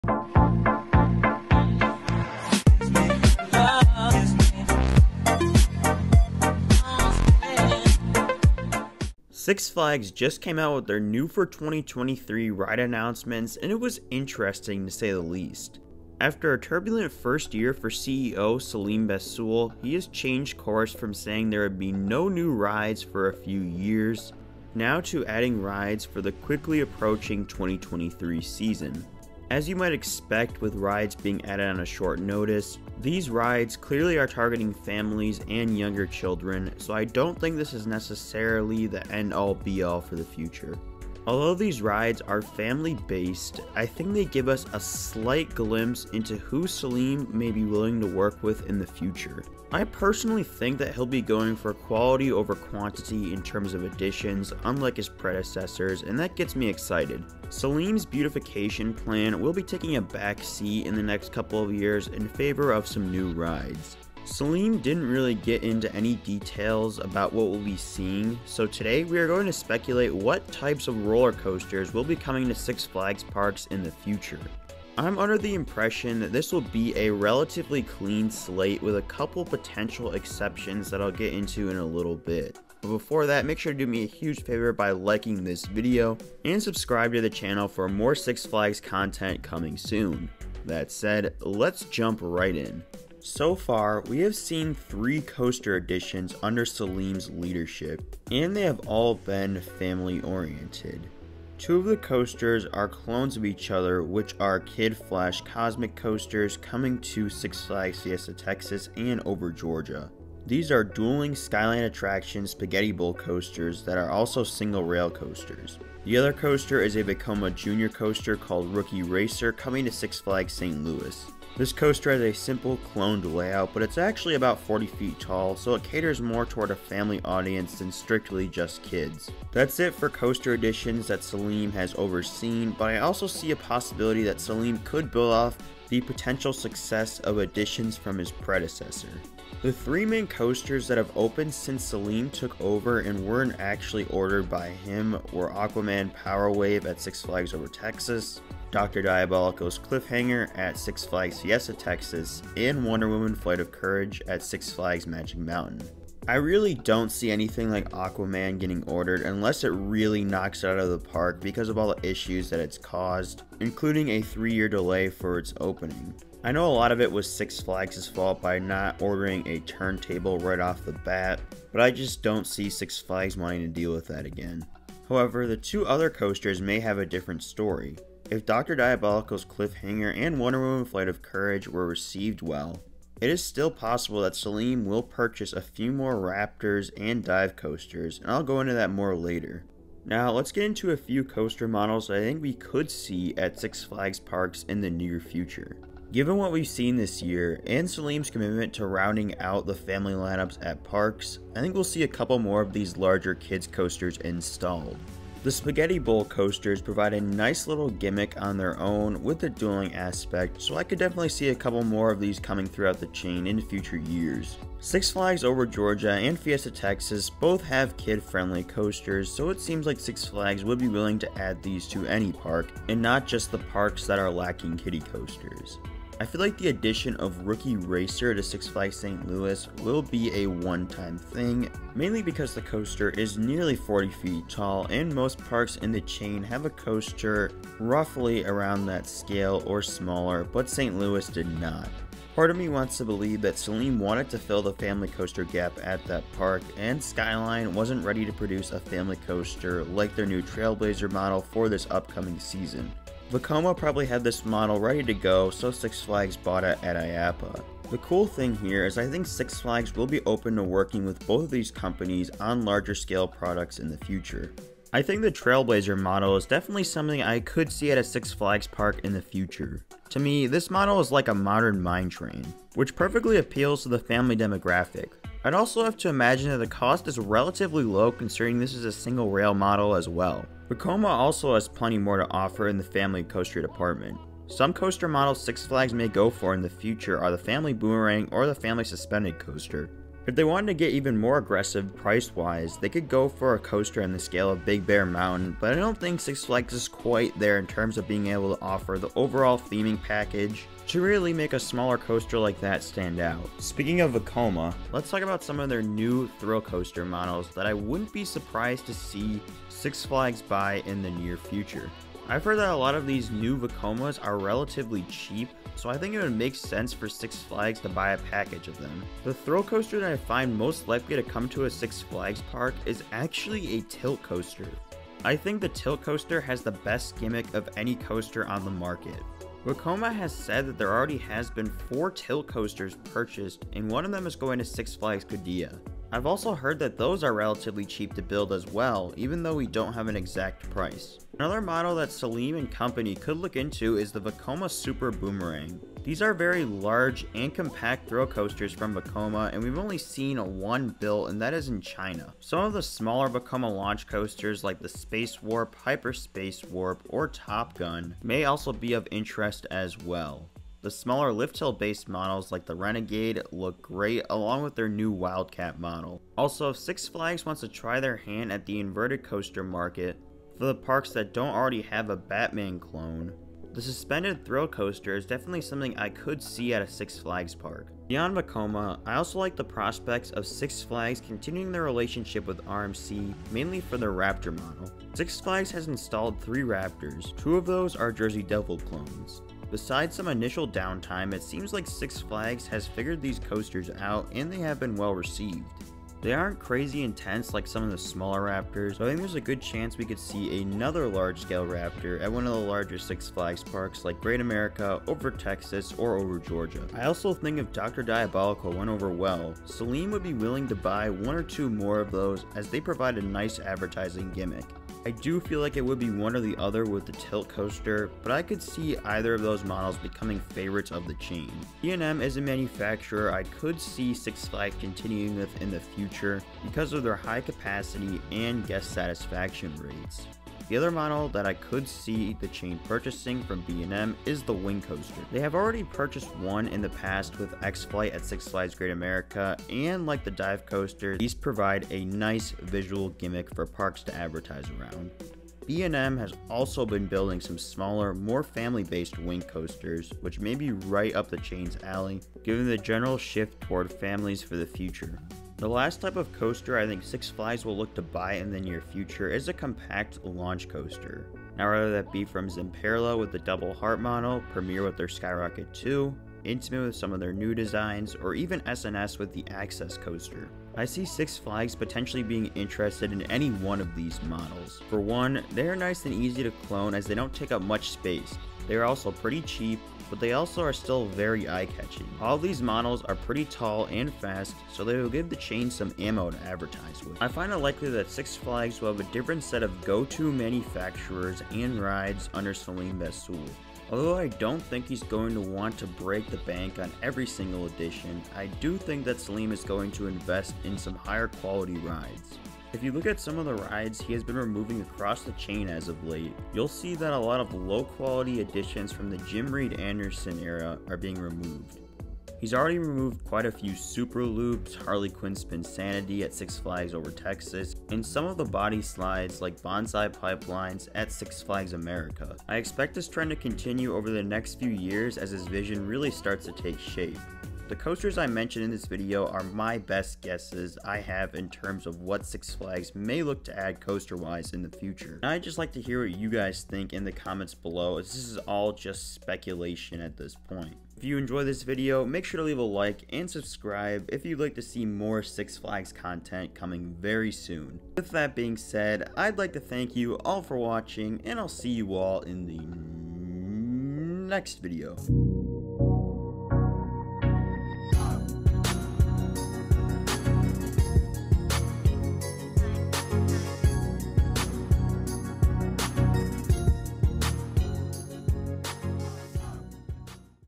The Future of Six Flags Coaster Additions
Six Flags just announced their new for 2023 additions, and since this is the first time we've seen CEO Selim Bassoul add rides, this has got me looking toward the future. I think Selim will take a different approach than past CEO, and I'm very interested to see what he comes up with. Today, I try to predict what kinds of coasters Six Flags may buy in future years for their parks.

It looks like you found our channel! If you like roller coasters or enjoy learning about them, you’ve come to the right place!